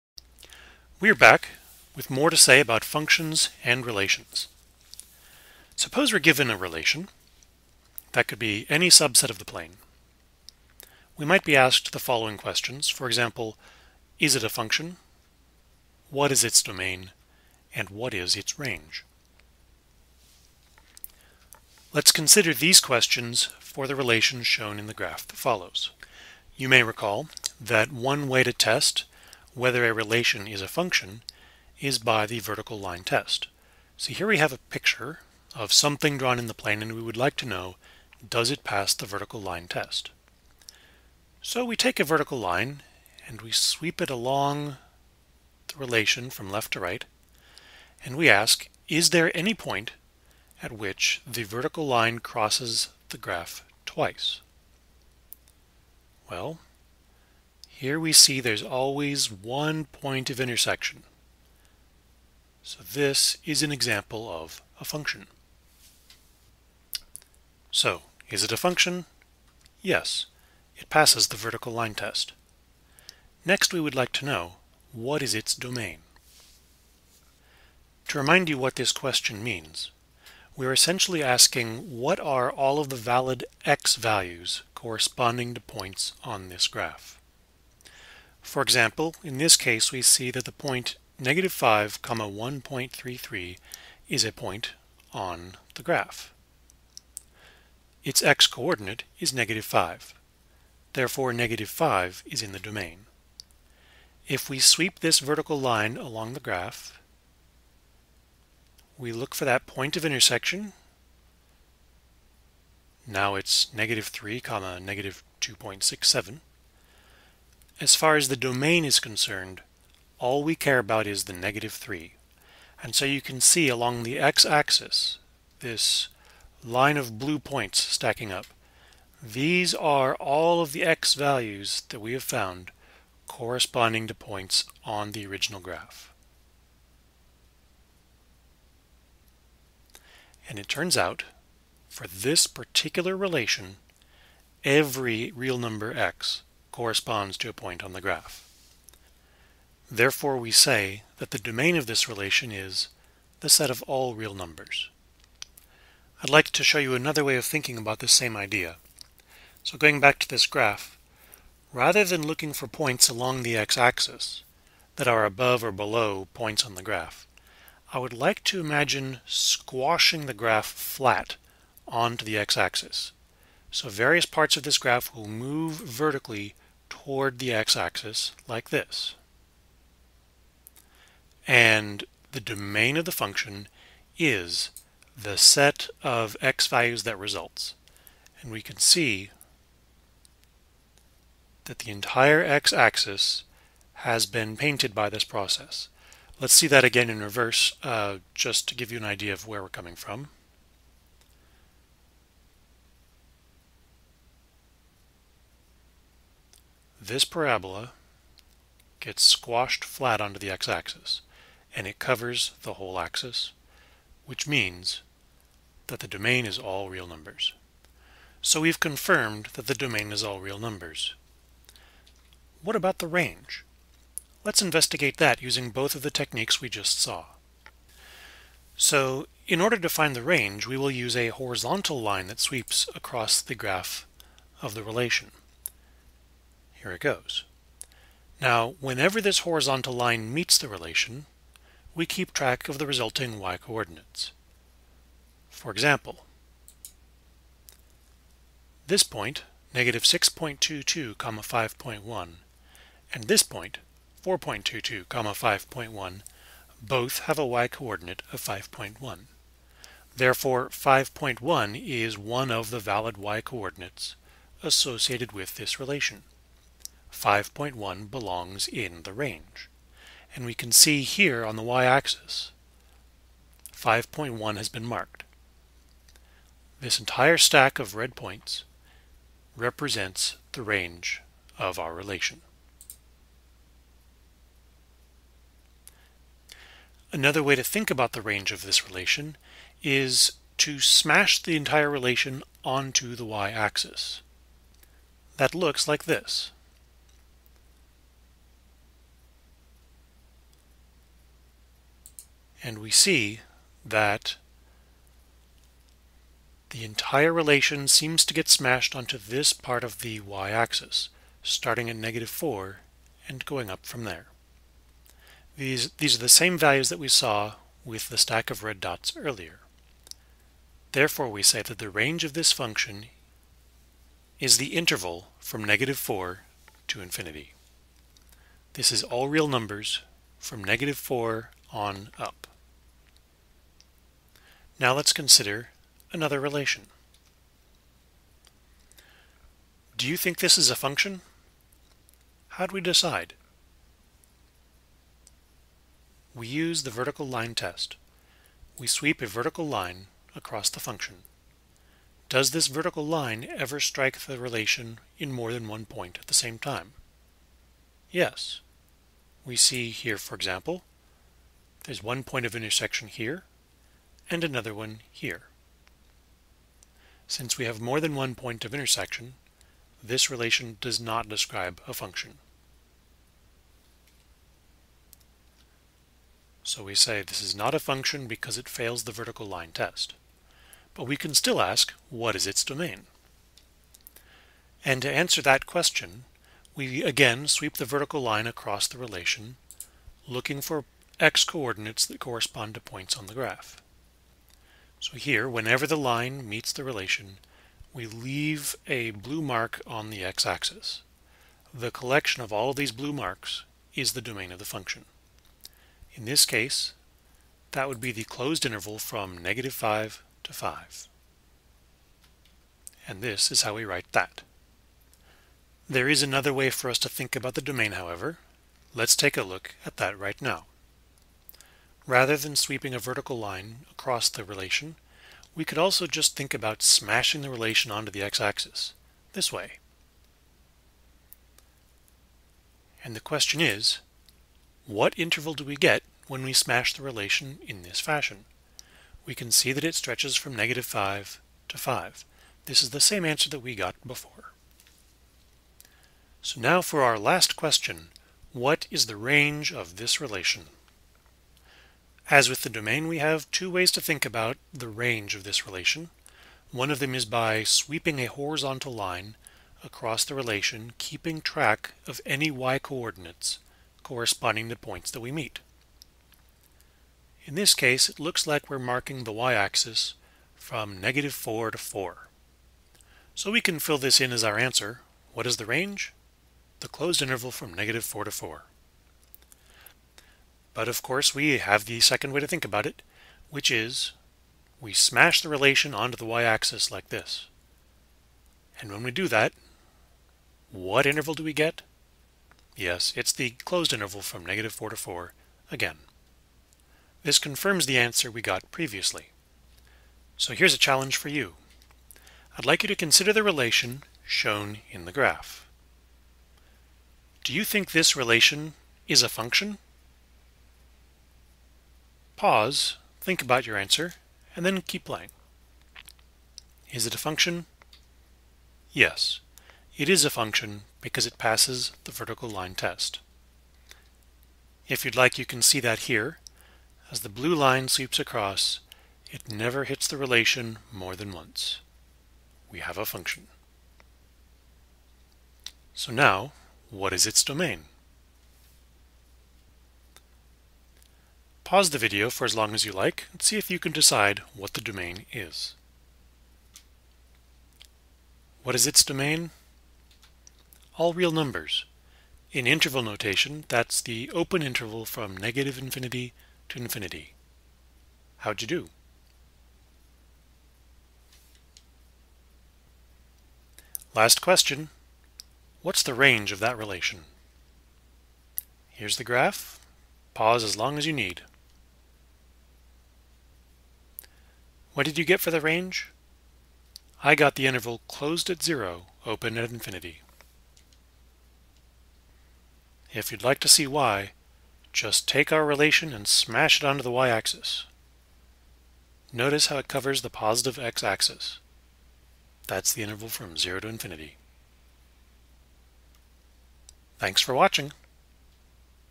Domain and Range of a Relation
How to find the Domain and Range of a relation from its graph